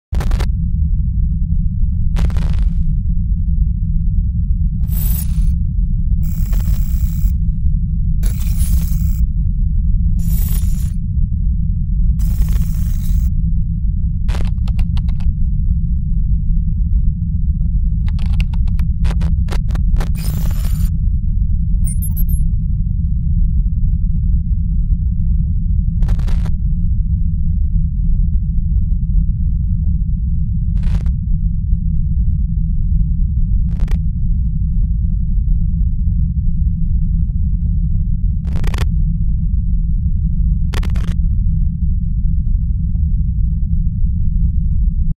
Videohive After Effects Project Files Free Download: Motion Text Maker
New MTscript : Easy text maker
5 style text animations
3 style elements (default 10 copies each design)
Change colors, animation offset, randomize, fonts size, tracking and more
Animate direction right-left or left-right writing (except Type A)
Text alignment Left, Right (except Type A)
Use any font
In and Out animation
No plugins required
Universalized expression (works with any AE’s language)
Video tutorial